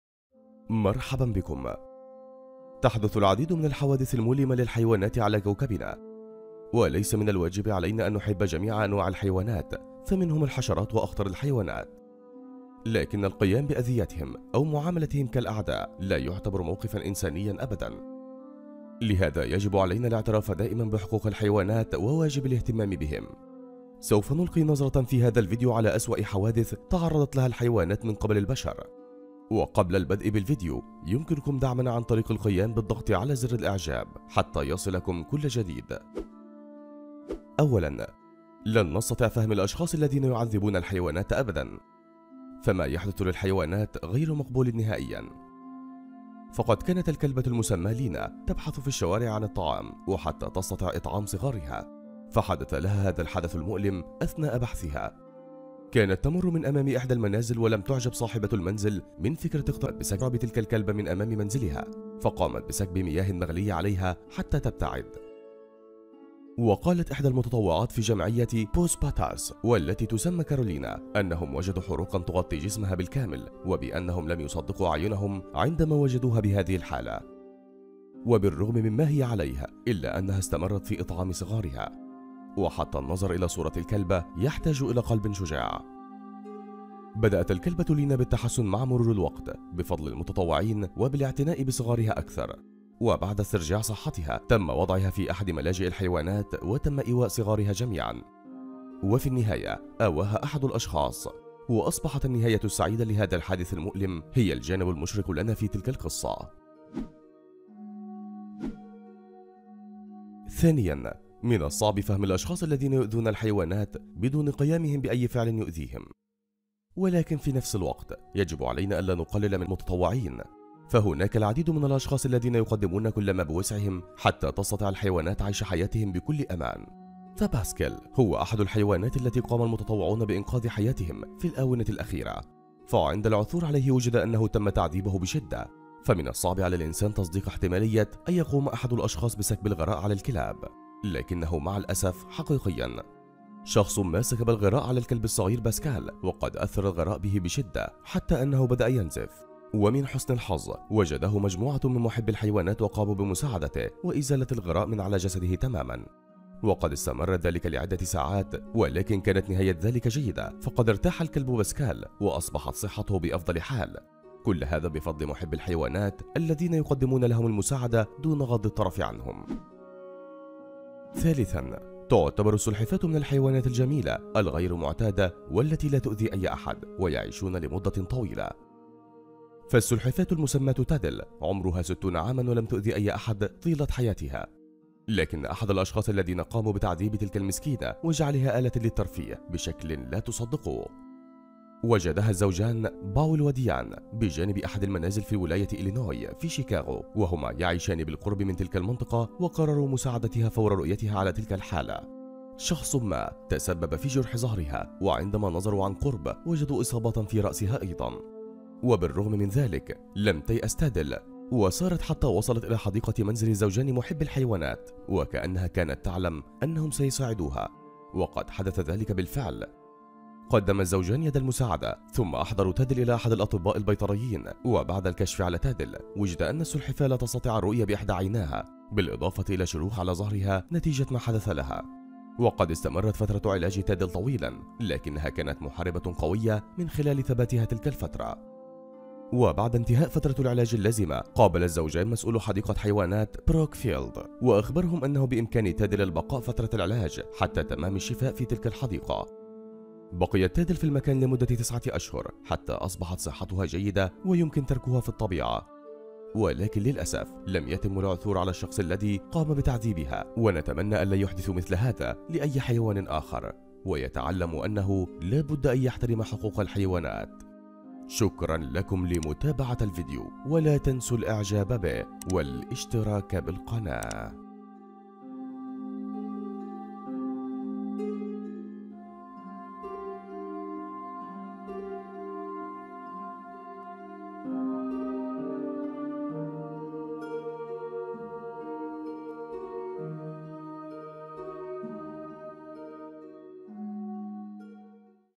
إعتداءات للبشر على الحيوانات - أسوأ ما يمكن أن تراه
تتعرض العديد من الحيوانات لحوادث مؤلمة من فعل البشر.
إيذاء الحيوانات ومعاملتها بشكل سيئ لا يعد موقفا إنسانياً أبداً. لهذا يجب علينا الاعتراف دائماً بحقوق الحيوانات وواجب الاهتمام بها.

أسوأ حوادث تعرضت لها الحيوانات من قِبل البشر:

1- أثناء بحث الكلبة "لينا" في الشوارع عن طعام، مرت صدفة من أمام منزل، حيث سكبت ربة هذا المنزل المياه المغلية عليها بحجة ابعادها، فأصيبت على الفور بحروق بالغة في كل انحاء جسدها، ورغم ذلك استمرت في اطعام صغارها.
اعتنى المتطوعون بالكلبة وصغارها، ومع مرور الوقت تحسن حال "لينا" ووضعت في أحد الملاجئ، كما تم ايواء صغارها.

2- "باسكال" هو احد الكلاب الذين أنقذهم المتطوعون في الآونة الأخيرة، فعند العثور عليه تبين أنه تم تعذيبه بشدة، إذ قام أحد الأشخاص بسكب الغراء عليه، فبدأ ينزف، الا ان مجموعة من محبي الحيوانات ساعدته عبر ازالة الغراء عن جسده تماماً، وقد استمر ذلك لساعات عدة.

3- السلحفاة المسماة "تادل" بولاية إلينوي في شيكاغو عمرها 60 عاماً ، بالغ احد الأشخاص في تعذ يبها مسببا لها جرحا بليغا في ظهرها وإصابات عدة في رأسها، ولحسن حظها وجدها زوجان وقدما لها المساعدة.

نرجو دعمكم عن طريق الضغط على زر الإعجاب حتى يصلكم كل جديد.